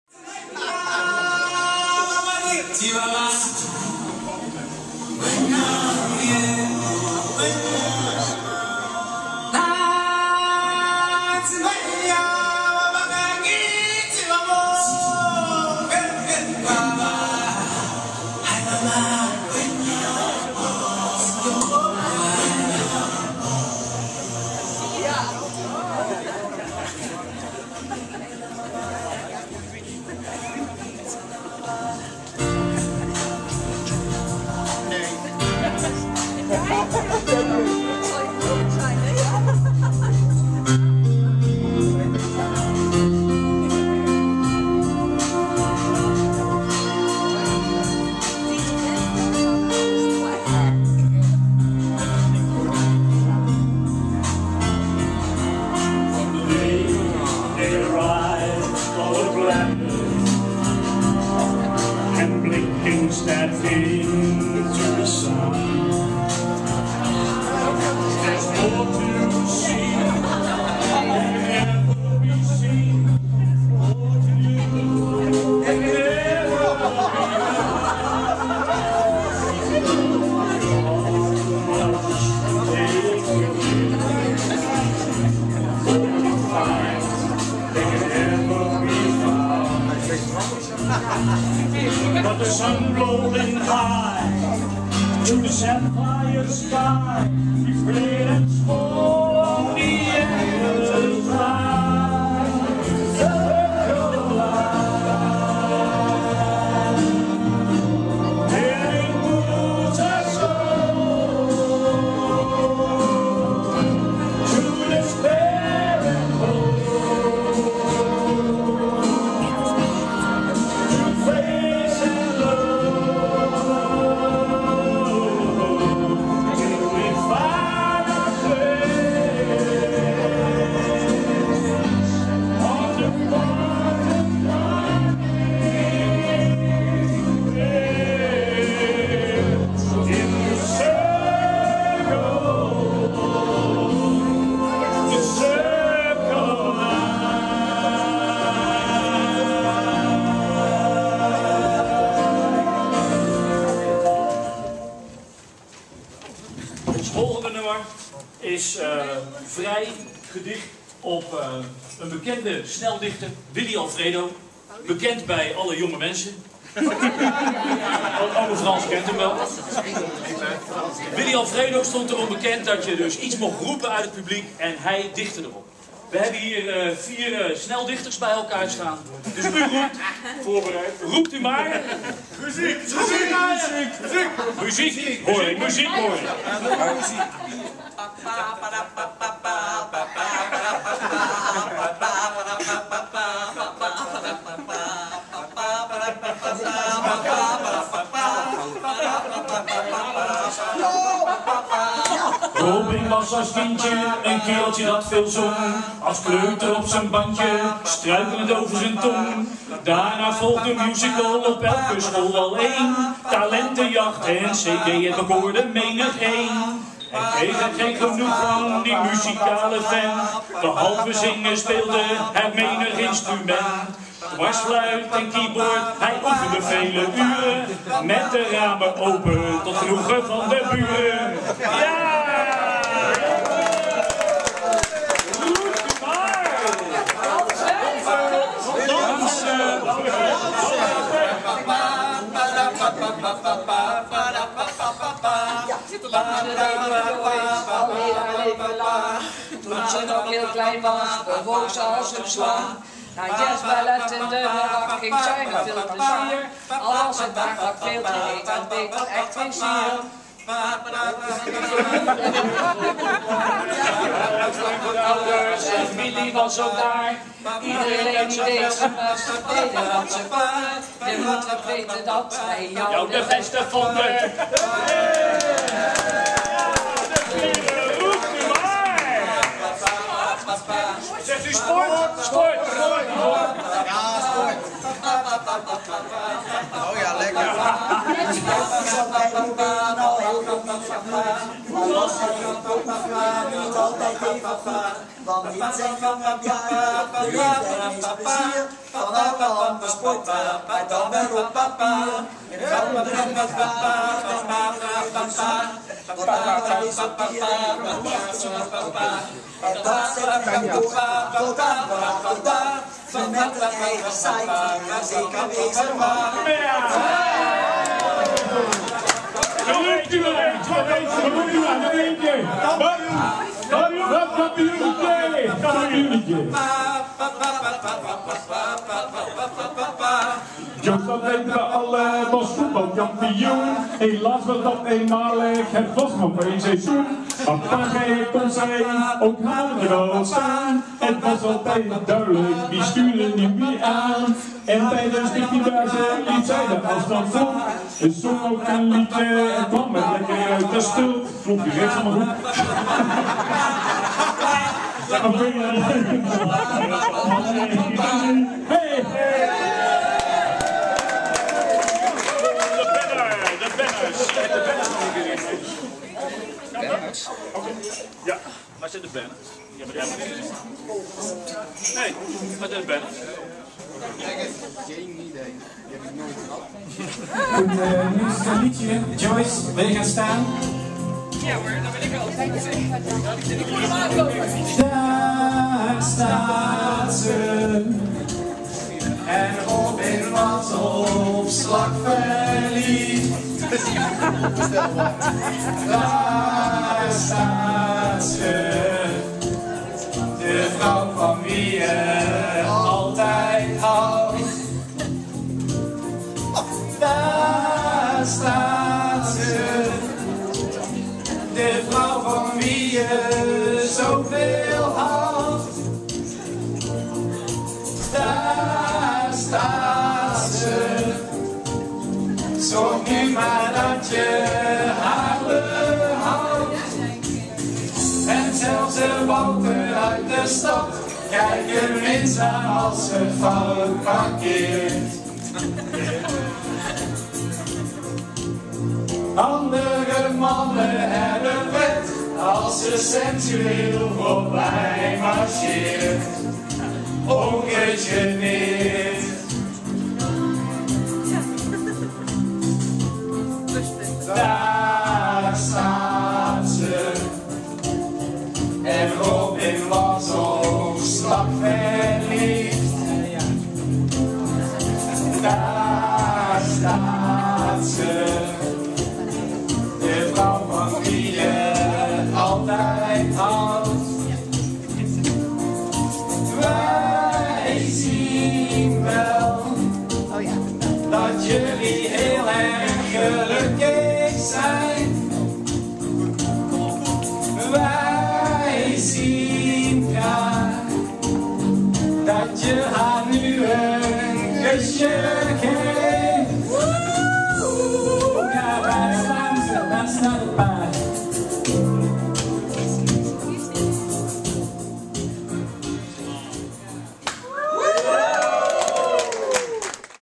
29 juni 2012 - R&J De Musical (deel 1)
Op onze trouwdag bracht een unieke cast van vrienden en familie 'R&J De Musical' met muzikale hoogtepunten uit ons leven. Een absolute kaskraker!